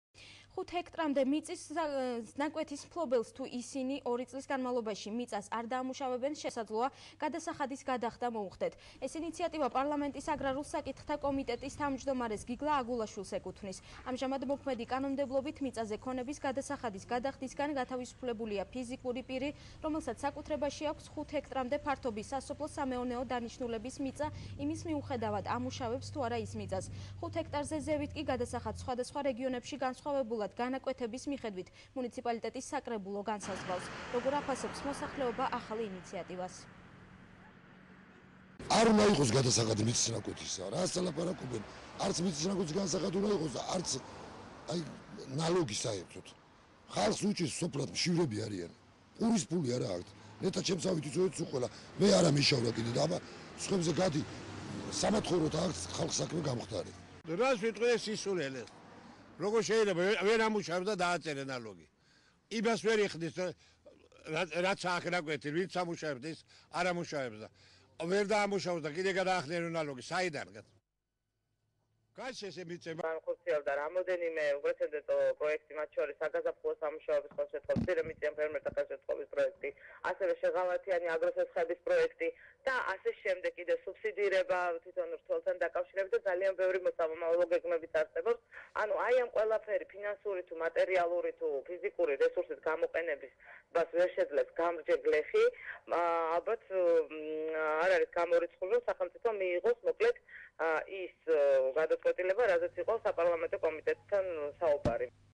აგრარულ საკითხთა კომიტეტის ინიციატივა
5 ჰექტრამდე მიწის ნაკვეთის მფლობელებს, თუ ისინი 2 წლის განმავლობაში მიწას არ დაამუშავებენ, შესაძლოა, გადასახადის გადახდა მოუხდეთ. ეს ინიციატივა პარლამენტის აგრარულ საკითხთა კომიტეტის თავმჯდომარეს, გიგლა აგულაშვილს ეკუთვნის. ამჟამად მოქმედი კანონმდებლობით, მიწაზე ქონების გადასახადის გადახდისგან განთავისუფლებულია ფიზიკური პირი, რომელსაც საკუთრებაში აქვს 5 ჰექტრამდე ფართობის სასოფლო-სამეურნეო დანიშნულების მიწა, იმის მიუხედავად, ამუშავებს თუ არა ის მიწას. 5 ჰექტარზე ზევით კი, გადასახადს სხვადასხვა რეგიონებში განსხვავებულად, განაკვეთების მიხედვით, მუნიციპალიტეტის საკრებულო განსაზღვრავს. როგორ აფასებს მოსახლეობა ახალ ინიციატივას?